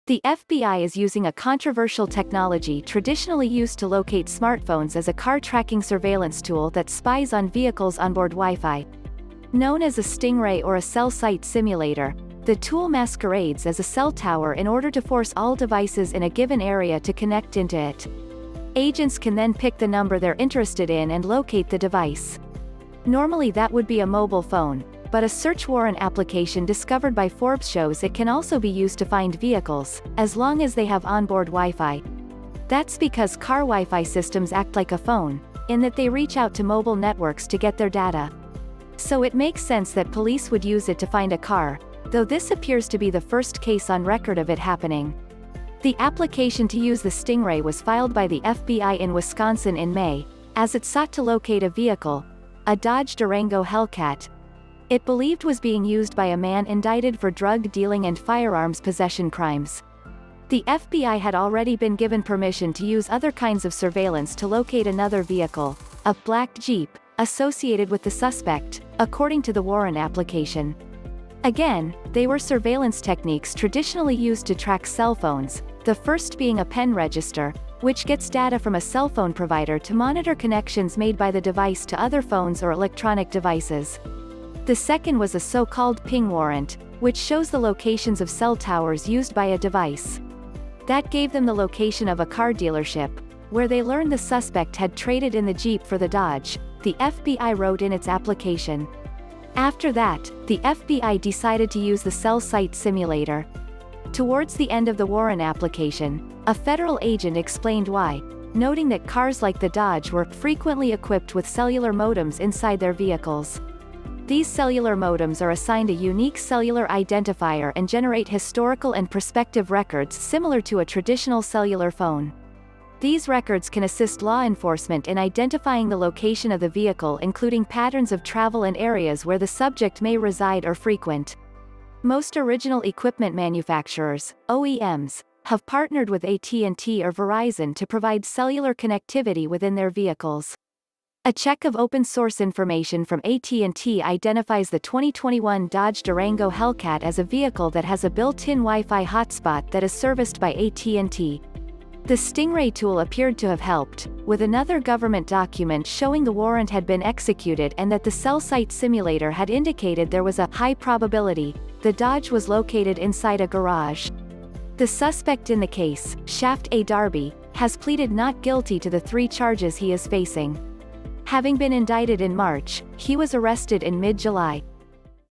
The FBI Is Locating Cars By Spying On Their WiFi
The FBI is using a controversial technology traditionally used to locate smartphones as a car tracking surveillance tool that spies on vehicles’ on-board WiFi. Known as a Stingray or a cell-site simulator, the tool masquerades as a cell tower in order to force all devices in a given area to connect into it. Agents can then pick the number they’re interested in and locate the device. Normally that would be a mobile phone, but a search warrant application discovered by Forbes shows it can also be used to find vehicles, as long as they have onboard Wi-Fi. That’s because car Wi-Fi systems act like a phone, in that they reach out to mobile networks to get their data. So it makes sense that police would use it to find a car, though this appears to be the first case on record of it happening. The application to use the Stingray was filed by the FBI in Wisconsin in May, as it sought to locate a vehicle - a Dodge Durango Hellcat - it believed was being used by a man indicted for drug dealing and firearms possession crimes. The FBI had already been given permission to use other kinds of surveillance to locate another vehicle, a “black Jeep,” associated with the suspect, according to the warrant application. Again, they were surveillance techniques traditionally used to track cellphones, the first being a pen register, which gets data from a cellphone provider to monitor connections made by the device to other phones or electronic devices. The second was a so-called “ping warrant,” which shows the locations of cell towers used by a device. That gave them the location of a car dealership, where they learned the suspect had traded in the Jeep for the Dodge, the FBI wrote in its application. After that, the FBI decided to use the cell-site simulator. Towards the end of the warrant application, a federal agent explained why, noting that cars like the Dodge were “frequently equipped with cellular modems inside their vehicles. These cellular modems are assigned a unique cellular identifier and generate historical and prospective records similar to a traditional cellular phone.”“These records can assist law enforcement in identifying the location of the vehicle including patterns of travel and areas where the subject may reside or frequent. Most Original Equipment Manufacturers (OEMs) have partnered with AT&T or Verizon to provide cellular connectivity within their vehicles. A check of open source information from AT&T identifies the 2021 Dodge Durango Hellcat as a vehicle that has a built-in WiFi hotspot that is serviced by AT&T.”The Stingray tool appeared to have helped, with another government document showing the warrant had been executed and that the cell-site simulator had indicated there was a “high probability” the Dodge was located inside a garage. The suspect in the case, Shaft A. Darby, has pleaded not guilty to the three charges he is facing. Having been indicted in March, he was arrested in mid-July.